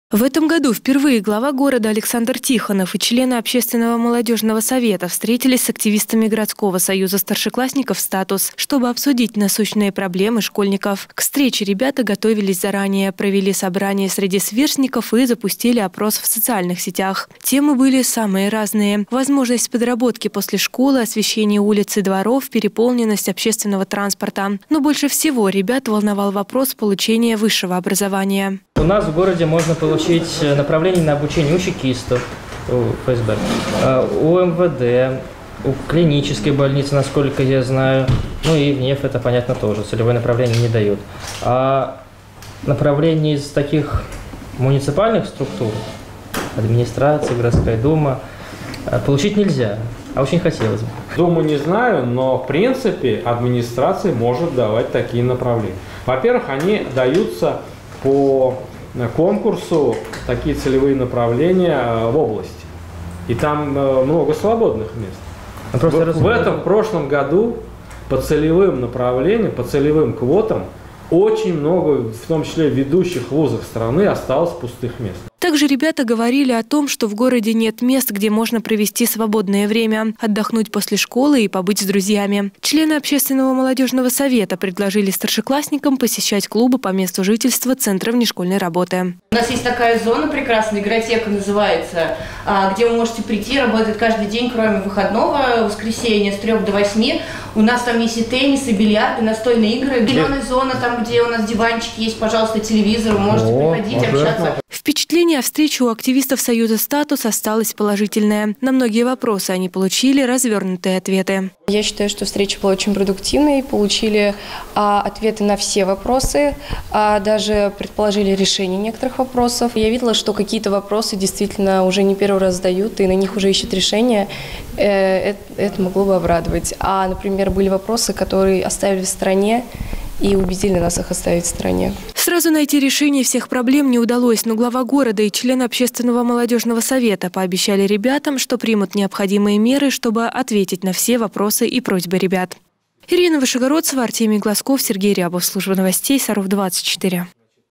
Встреча главы города и активистов союза «СтаТУС»
Активисты союза старшеклассников «СтаТУС» встретились с членами молодёжного совета при главе города. Школьники поделились проблемами и смогли задать Александру Тихонову интересующие их вопросы.